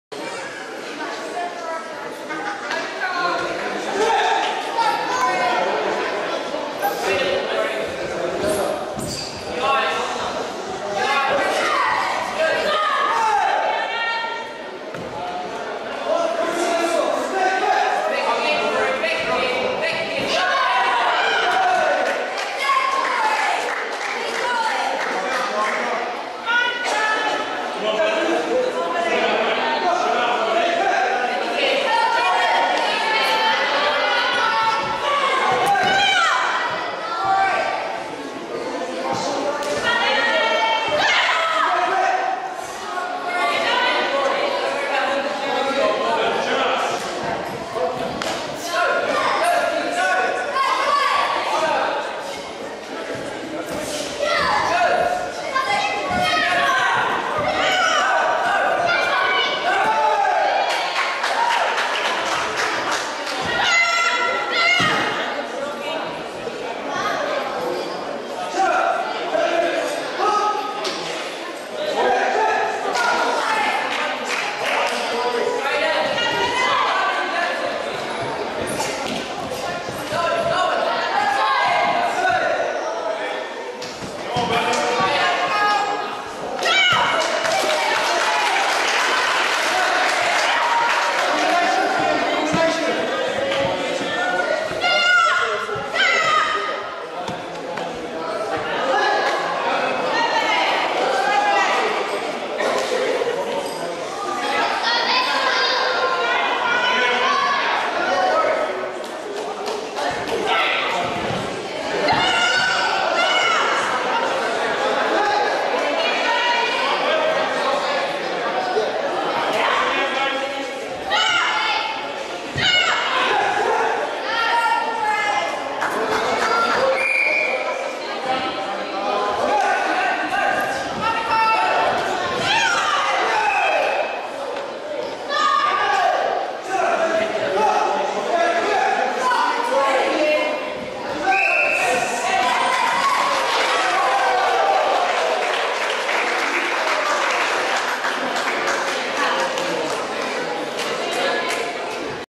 wrka kyoso cup 2017 11 04 rg2 division f16 19yrs+ female intermediate-advanced kumite 05a
2017 WRKA Kyōsō Cup Karate Championships

04 November 2017

Ring2: Division F16 19yrs+ Female Intermediate/Advanced Kumite